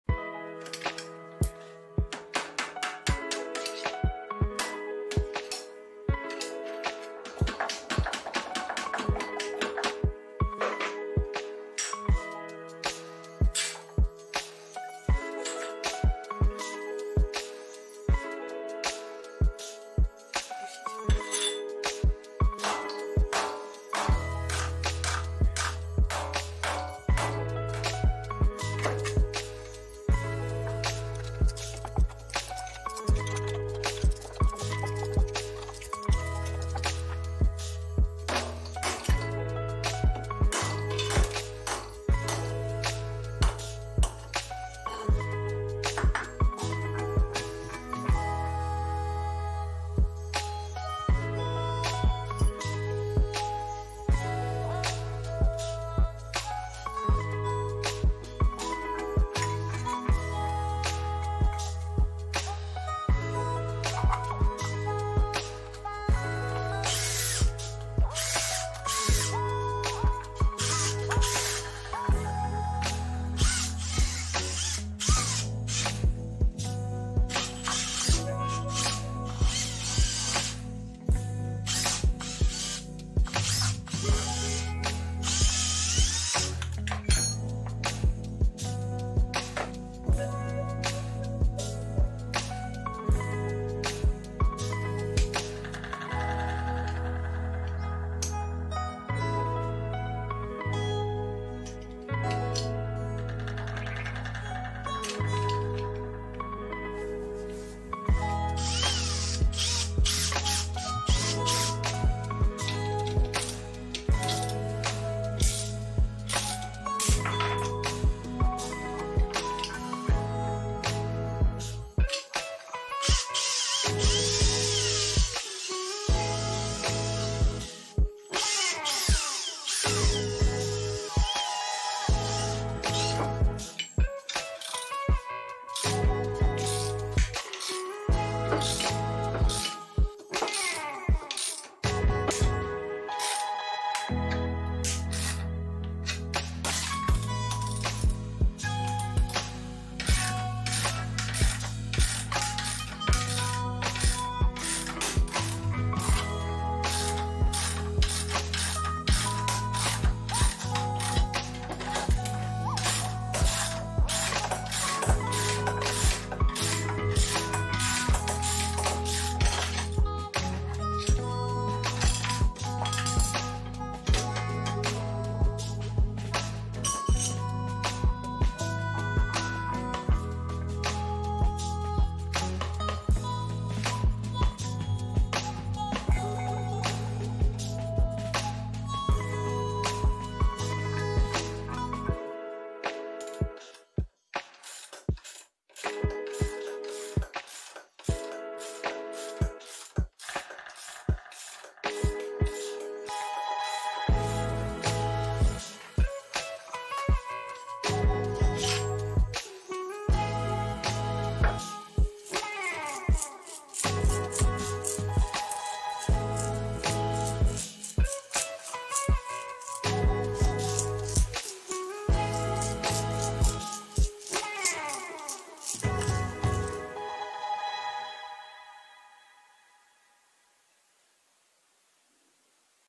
Removing mini lathe spindle bearings
Mini lathes tend to have different spindles and bearing types, my WM210V turned out to have 32010 which are 50x80x20mm. Will probably replace them with NSK bearings of the same type.